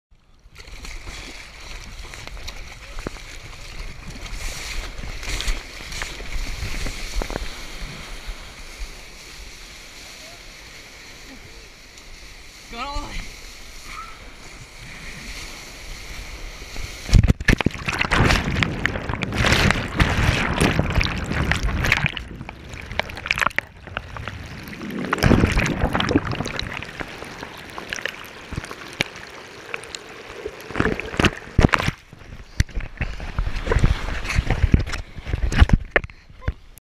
Cocoa Beach Surf Day GoPro Test
Testing the GoPro at Cocoa Beach, Fl. No nacho's at the end!